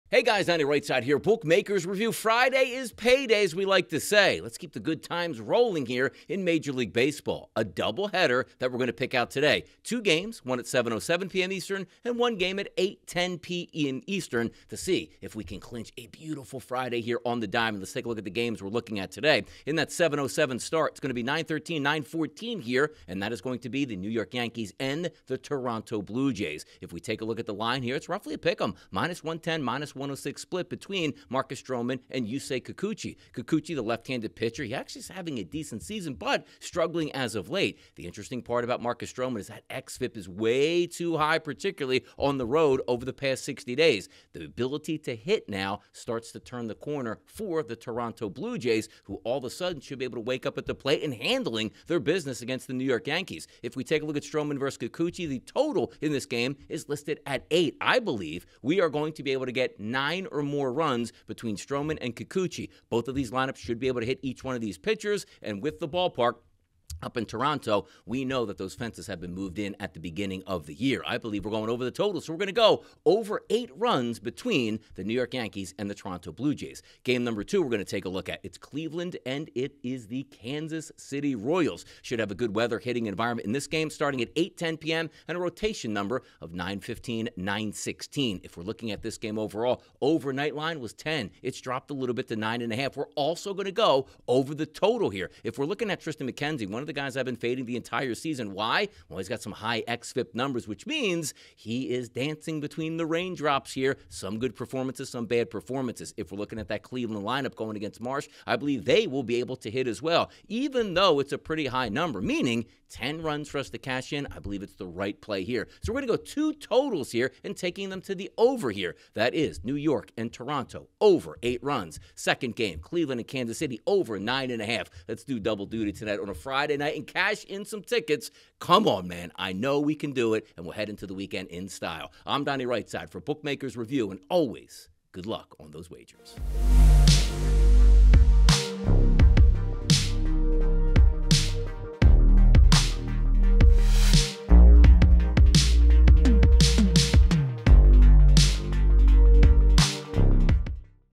MLB Picks | Analysis & Predictions by Donnie RightSide (June 28th)
Today’s MLB slate is full of value, and if you want to take advantage of it, you better check our MLB picks for Friday's games:

Yankees vs. Blue Jays 0:24
Guardians vs. Royals 1:21

Tune in as Donnie RightSide delivers his MLB analysis and predictions for the most valuable games of the day.